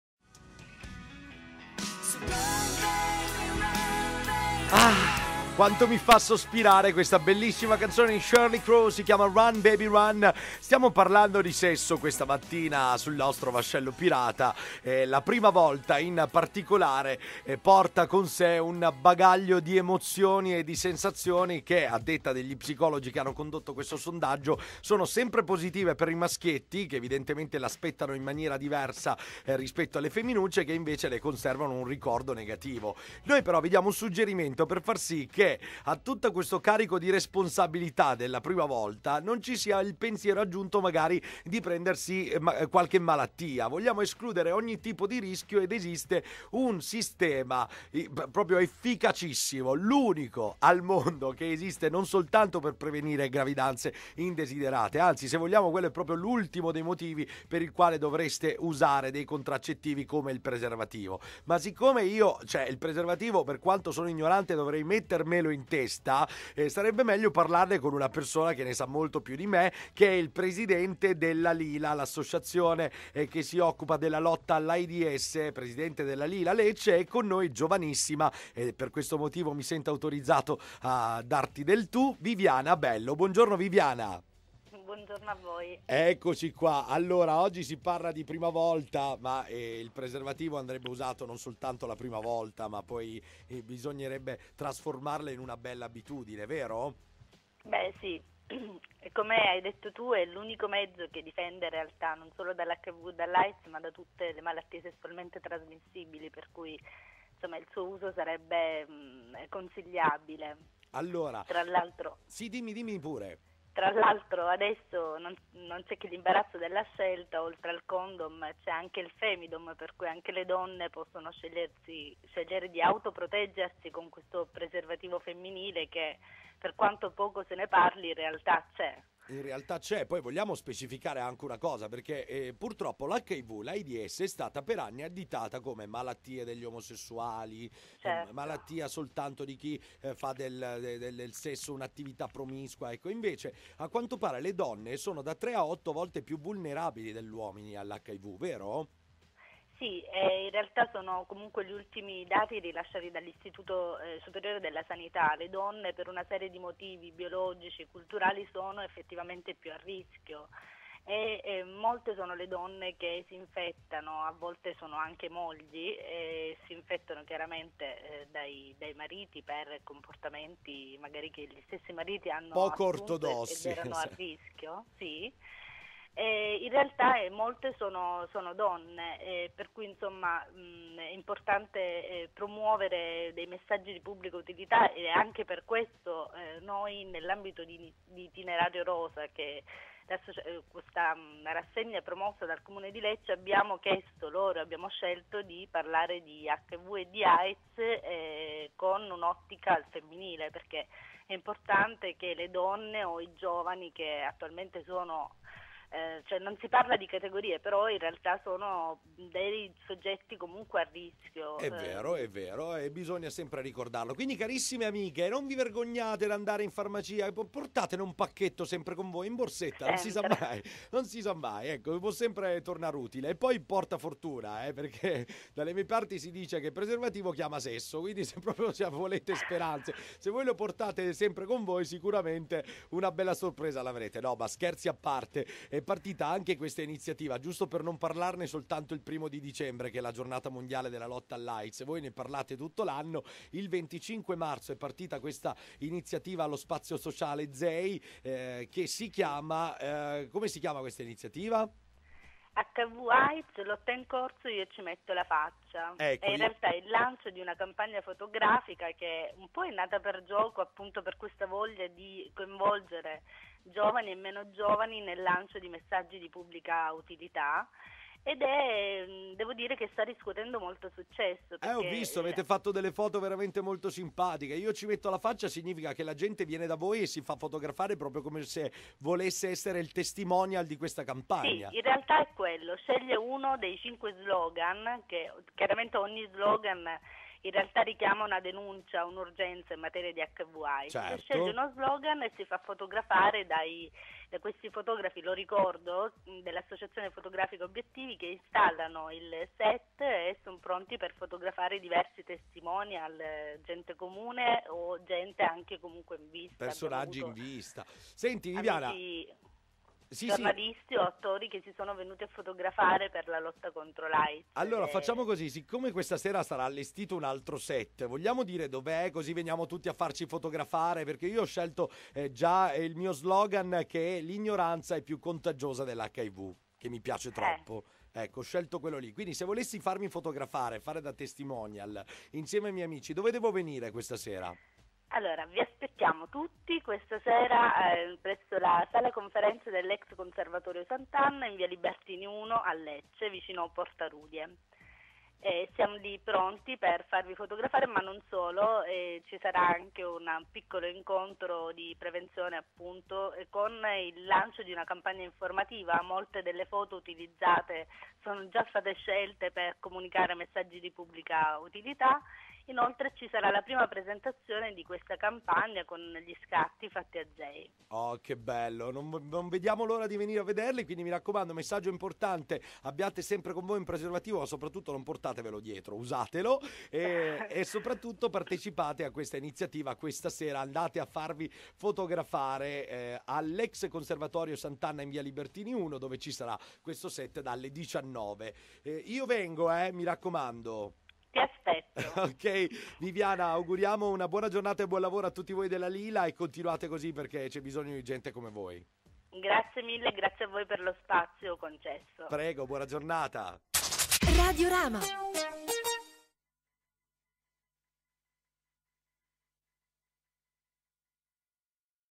RADIO RAMA - Intervista a VIVIANA BELLO Presidente LILA LECCE - 6 APRILE 2011
*la campagna "hiv/Aids? Lotta in corso. Io ci metto la faccia" è promossa da LILA LECCE | ASSOCIAZIONE FOTOGRAFICA OBIETTIVI | ZEI SPAZIO SOCIALE

 Dopo il successo della prima serata c/o lo Spazio Sociale ZEI il 25 marzo scorso, in cui ben 70 hanno scelto di metterci la faccia per la lotta contro l'Aids, la campagna continua con la ricerca di altri volti e altre facce. Ti aspettiamo il 6 aprile dalle 19.00!
Continua così la nostra sfida perchè.
...perchè non dobbiamo ricordarci dell'hiv/Aids solo nella giornata mondiale, il 1° dicembre. Perchè ogni giorno è un giorno in più per lottare contro l'Aids. Perchè la lotta all'Aids è qualcosa che riguarda tutti e ha bisogno di tutti. Perchè non è un "falso problema" come qualcuno dice o ancor meglio un problema inesistente. Perché è necessario informarsi, prendere una posizione, non solo scendendo in piazza ma "allo specchio. Non contro gli altri, ma contro noi stessi. Non con le marce o col chiasso degli slogan, ma nel silenzio della coscienza". Perchè occorre fare delle scelte consapevoli...